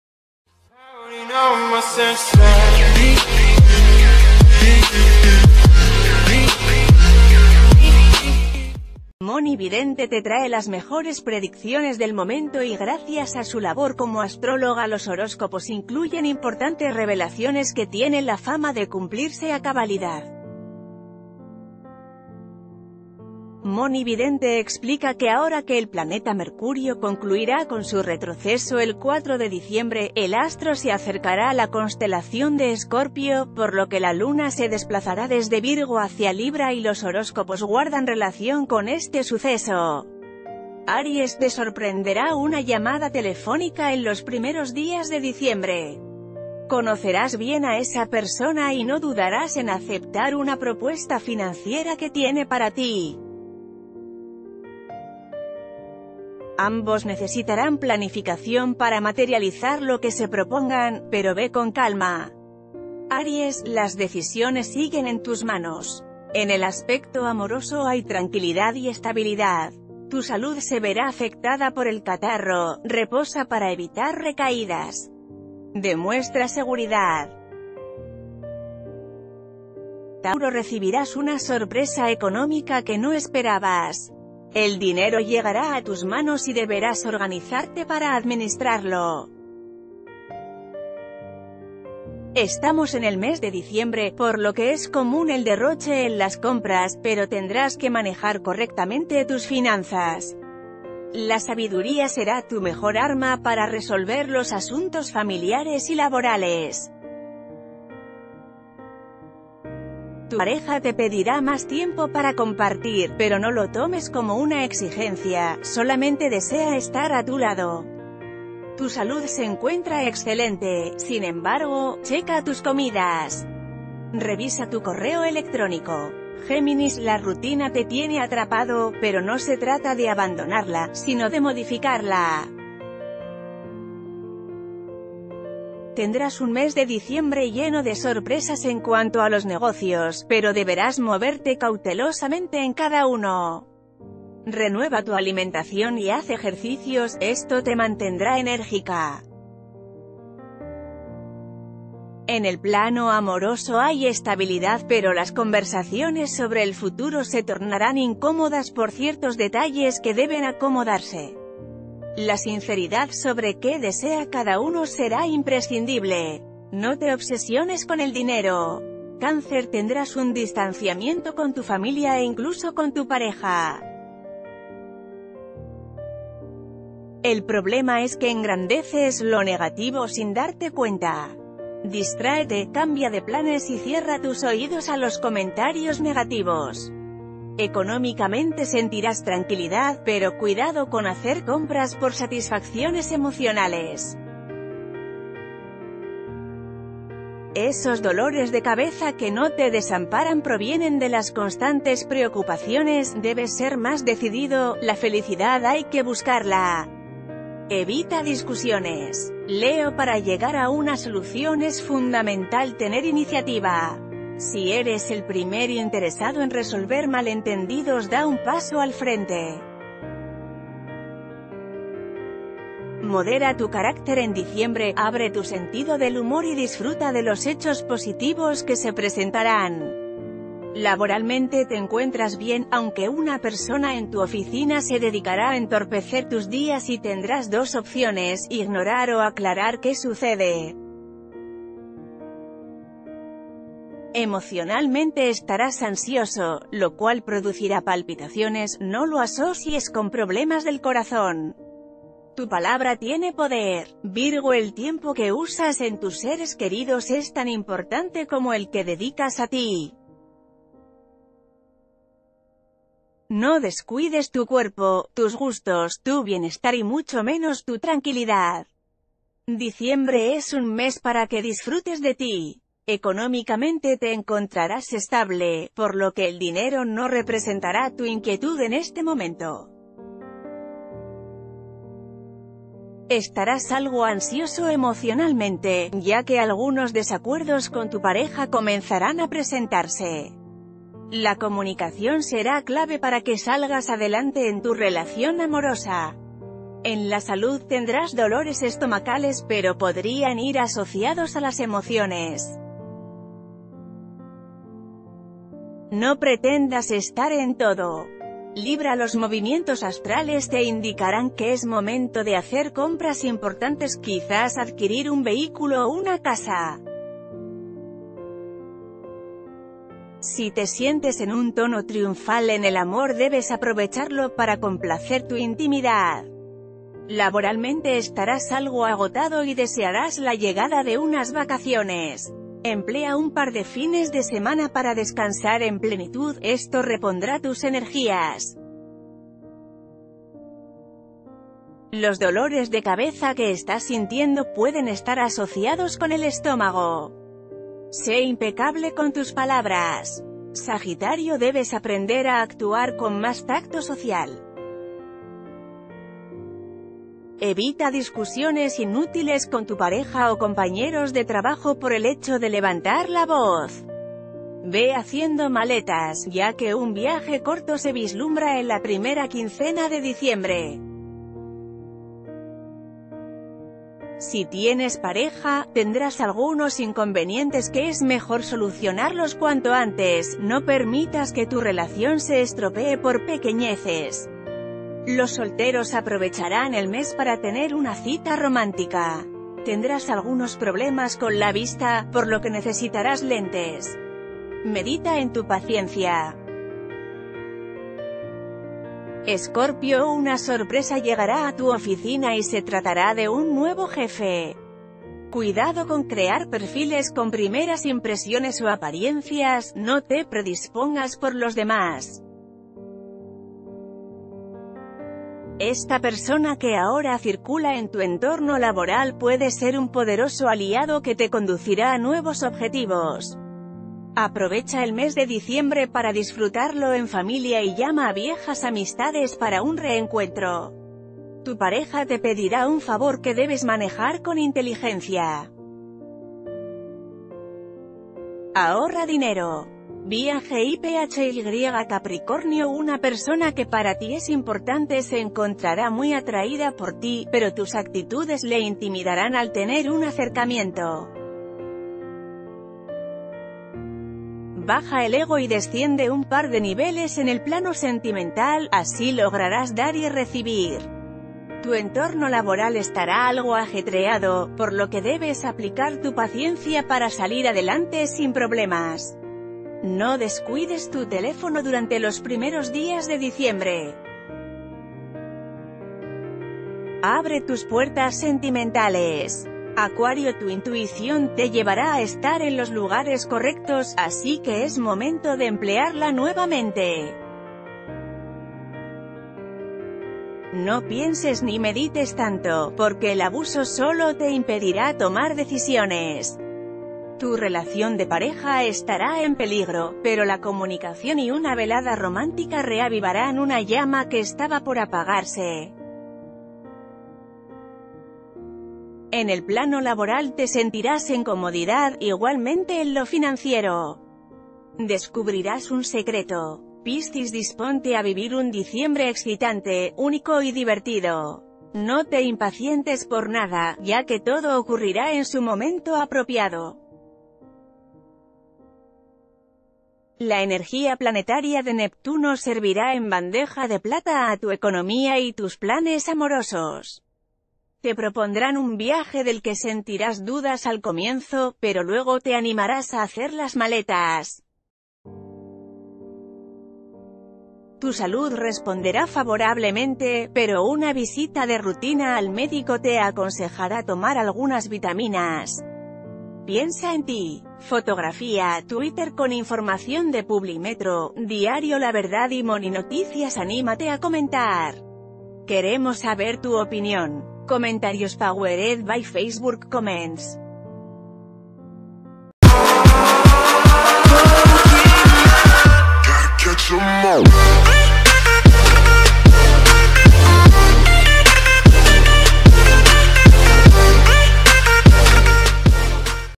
Mhoni Vidente lanza increíbles revelaciones decembrinas para cada signo zodiacal
Mhoni Vidente te trae las mejores predicciones del momento y gracias a su labor como astróloga los horóscopos incluyen importantes revelaciones que tienen la fama de cumplirse a cabalidad. Mhoni Vidente explica que ahora que el planeta Mercurio concluirá con su retroceso el 4 de diciembre, el astro ...